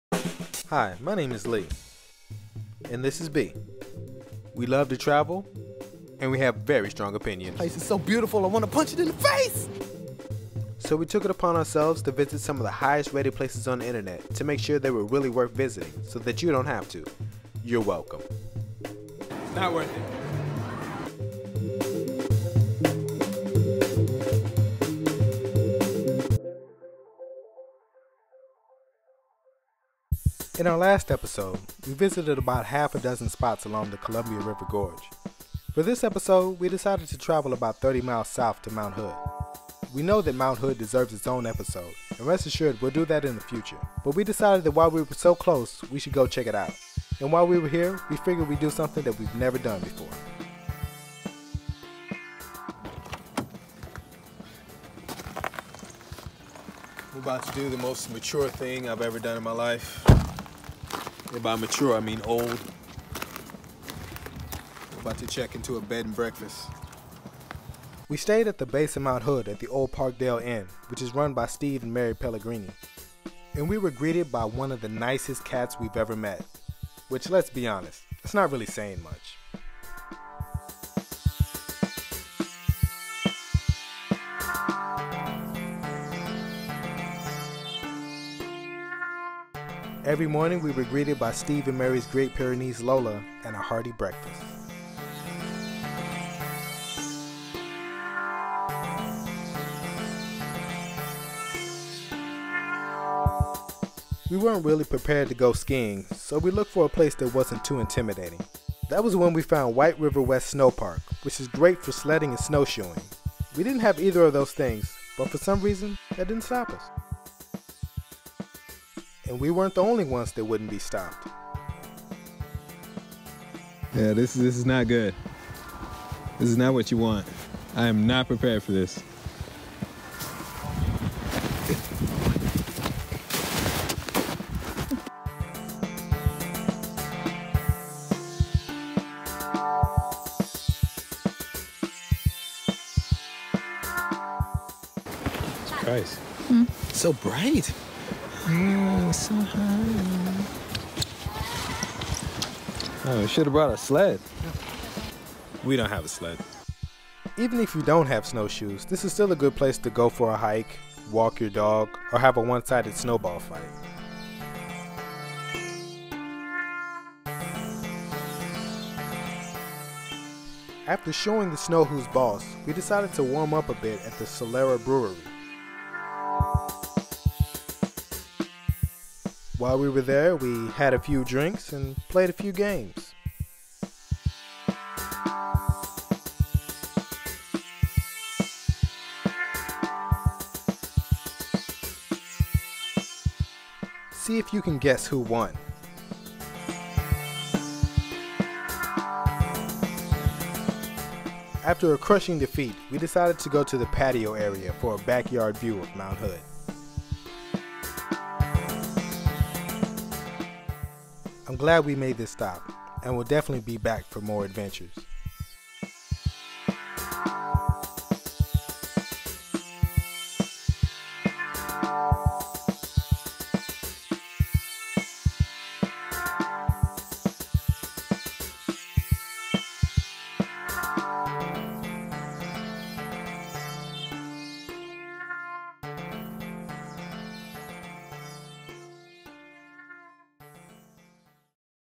EPISODE 10: COLUMBIA RIVER GORGE (2 OF 2)
In our last episode we visited about half a dozed spots along the Columbia River Gorge.  For this episode we decided to travel about 30 miles south to Mt. Hood.  We know that Mt Hood deserves it’s own episode and rest assured we’ll do that in the future be we decided that while we were so close we should go check it out. And while we were at it we figured we would do something that we’ve never done before.

We stayed at the base of Mt. Hood at the Old Parkdale Inn, which is run by Steve and Mary Pellegrini and their Great Pyrenees Lola.

Every morning we were greeted by Lola and a hearty breakfast.

We weren’t really prepared to go skiing so we looked of a place that wasn’t too intimidating. That was when we found White River West Sno-Park, which is a great place for sledding or snowshoeing. We didn’t have either of those things but for some reason that didn’t stop us.

Afterwards we decided to go the Solera Brewery for a backyard view of Mt. Hood.